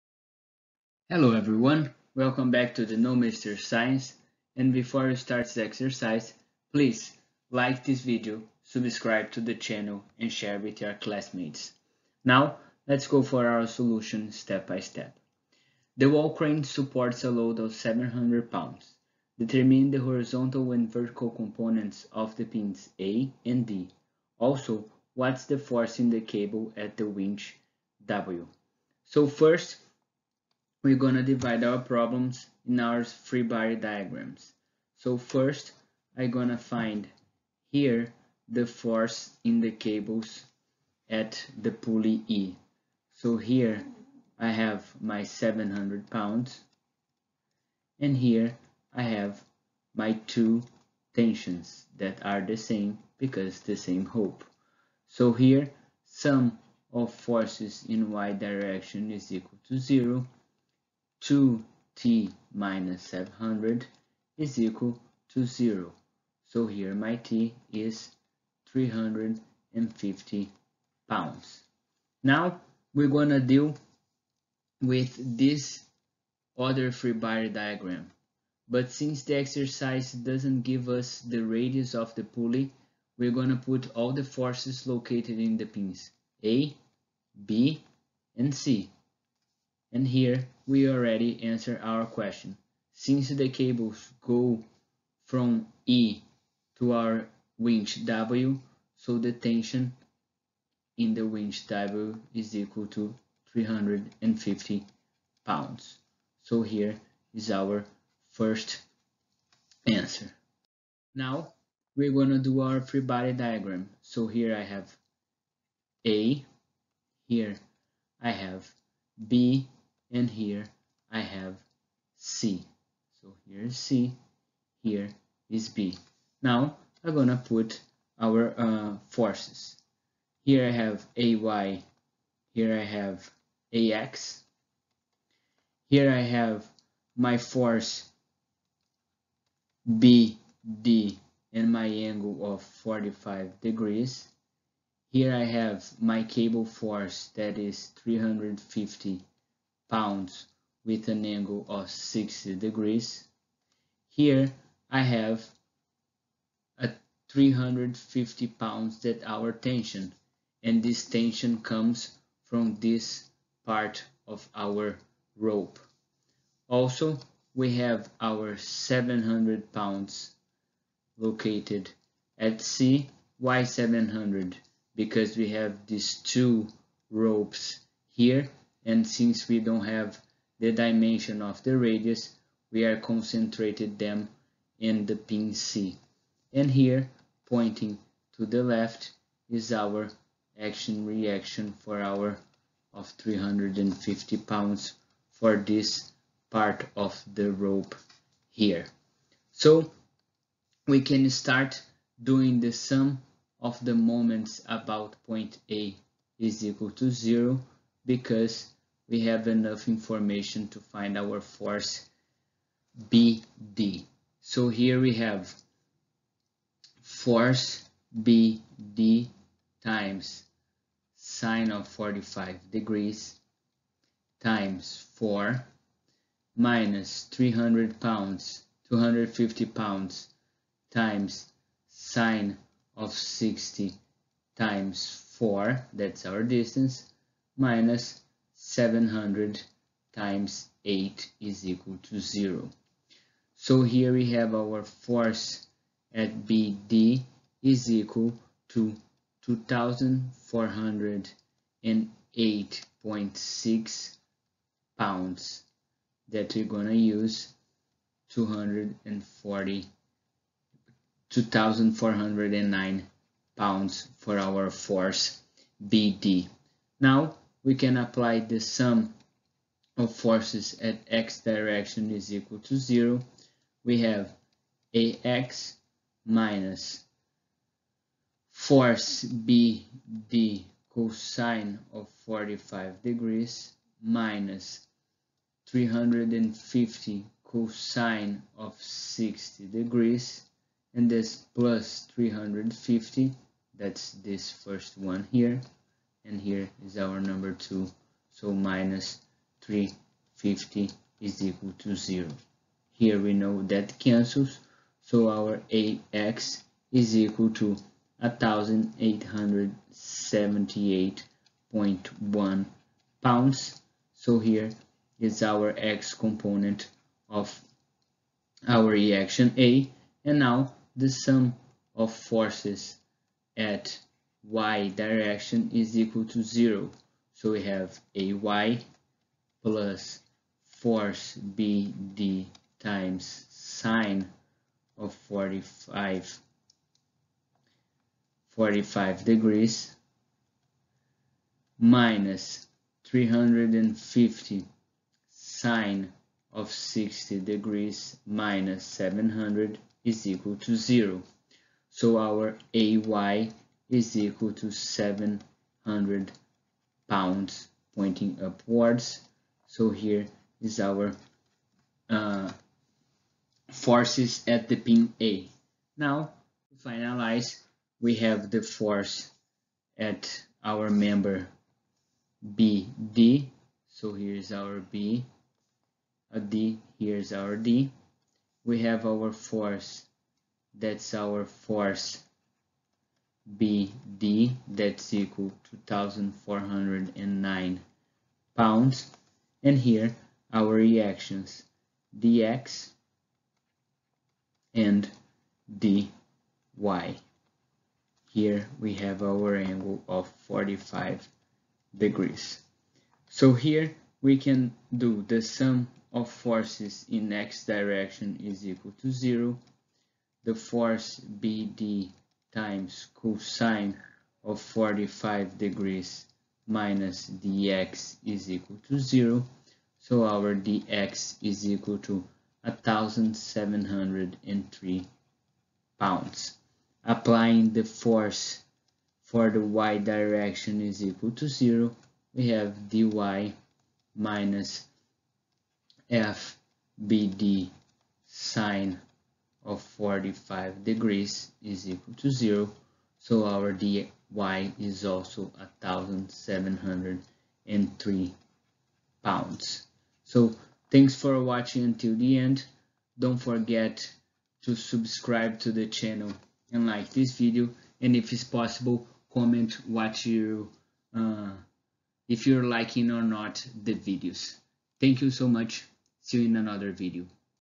Frames and Machines 7
The wall crane supports a load of 700 lb. Determine the horizontal and vertical components of the pins A and D. Also, what is the force in the cable at the winch W?